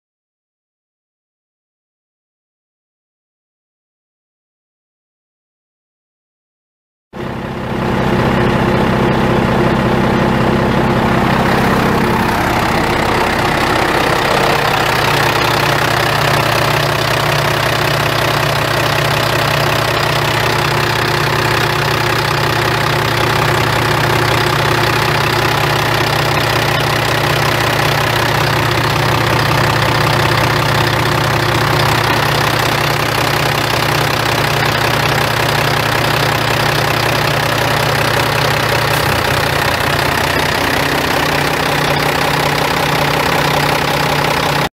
2001 Volvo VNL semi truck for sale | no-reserve Internet auction August 10, 2017
2001 Volvo VNL semi truck
139,535 miles on odometer
Volvo VE D12 12.1L L6 turbo diesel engine
Serial D12128603
Eaton Fuller RTL0-16610B ten speed manual transmission
Tandem axle
Cruise
AM/FM/cassette
Power windows
Heated mirrors
Air suspension seat
Sleeper
Single bed
Rear climate control
Hanging closet
Exterior visor
Dual exhaust stack
Quarter fenders
Air suspension
Air brakes
295/75R22.5 front tires
295/75R22.5 rear tires
275/80R22.5 rear tires
GVWR 50,350 lbs
Missouri title
4V4NC9GH01N256242
It's located in Eagleville, MO, and we're selling it to the highest bidder regardless of price.